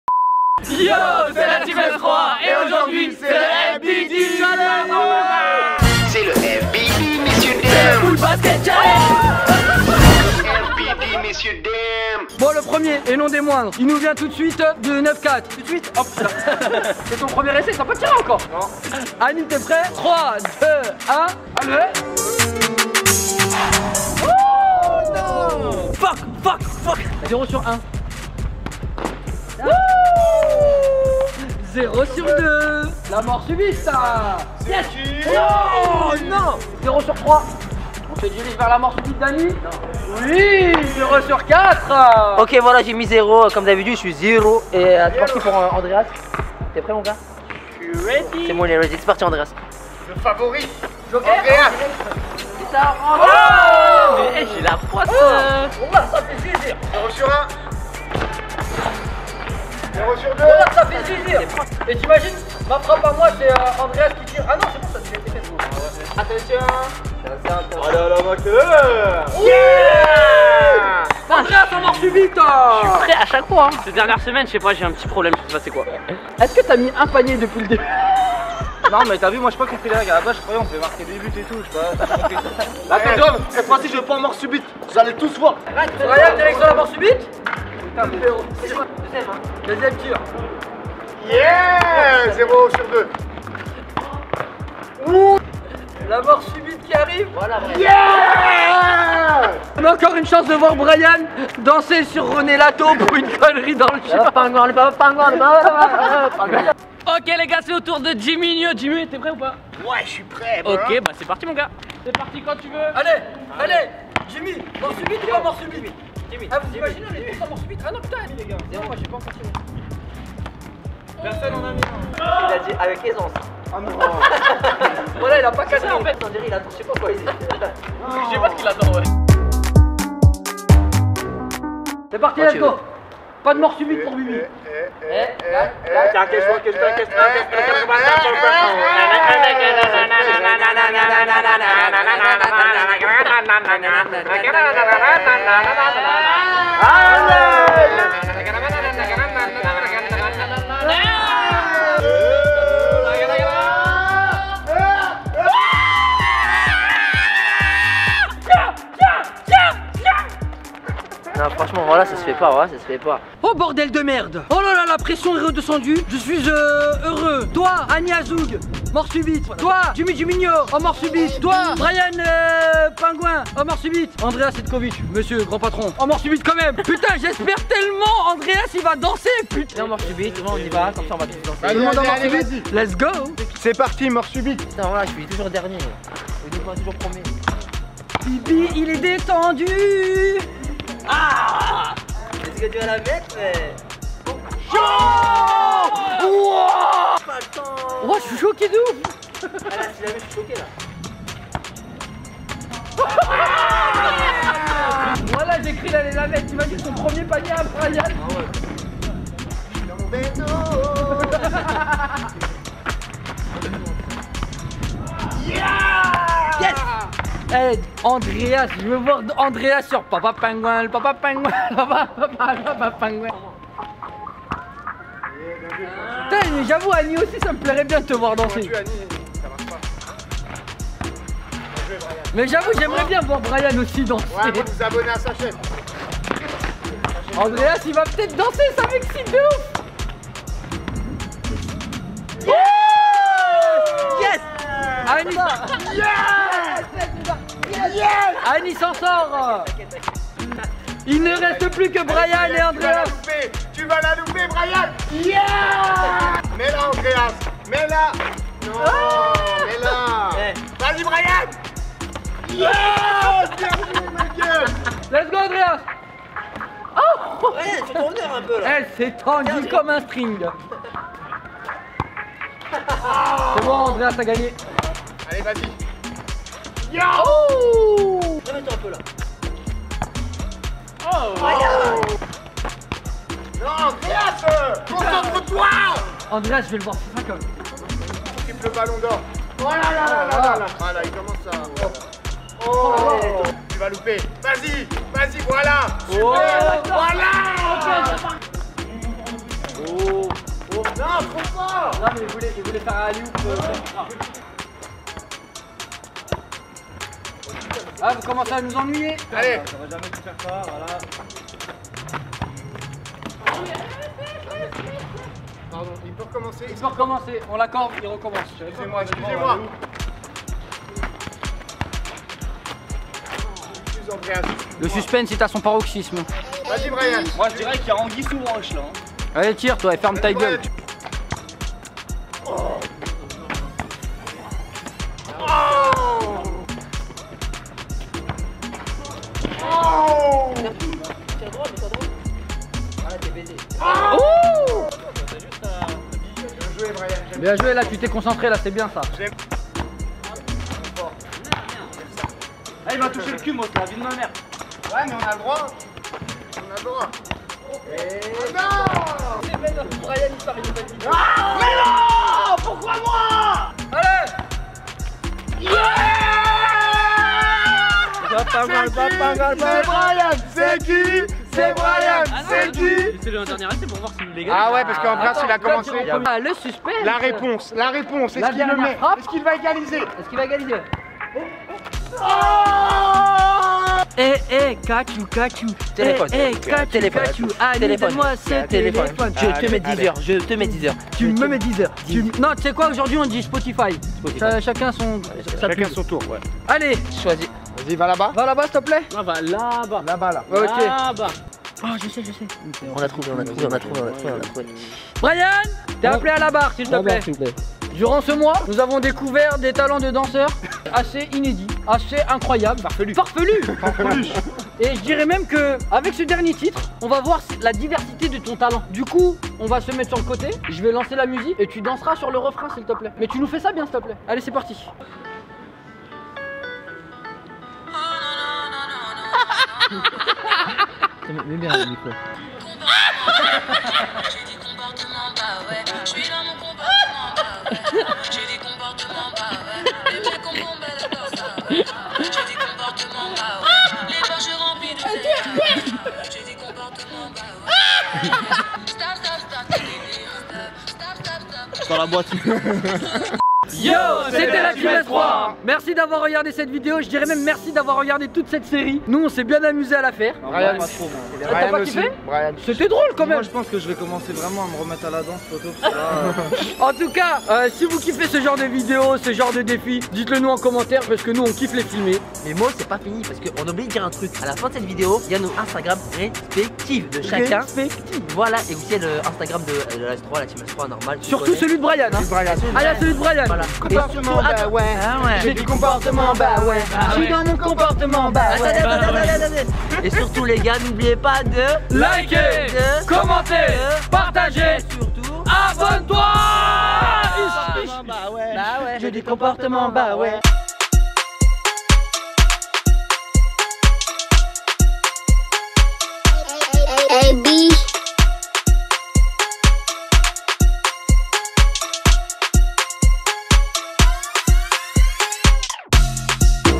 FBD CHALLENGE TEAM S3 EPISODE 4
Yo la S3 familles voila l'épisode 4 de notre série ( foot basket danse challenges) FBD ou le perdant s'affiche à danser sur dès son bien particulier